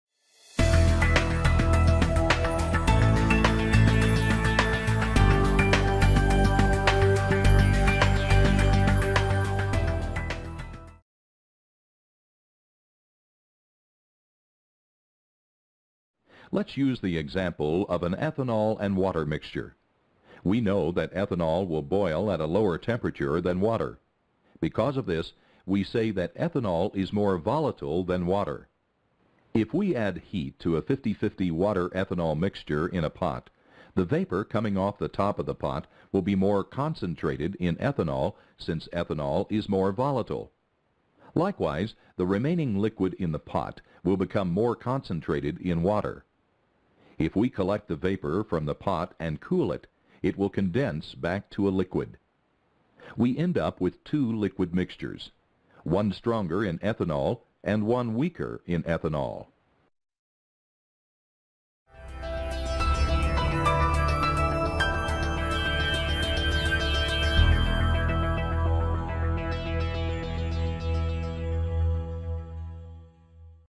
Distillation Basics Demonstration
New to the distillation process? Learn about distillation and how to condense a liquid mixture into two, one more concentrated in water and one more concentrated in ethanol.